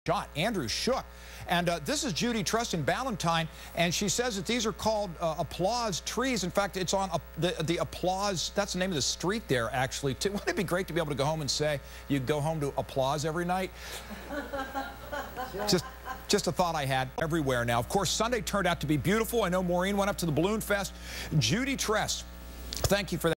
Fall Folliage and Hot Air Balloons
Fall Folliage on our street.  Trees called Autumn Applause ... Hot Air Balloon Festival in Statesville ... appearing on WBTV CBS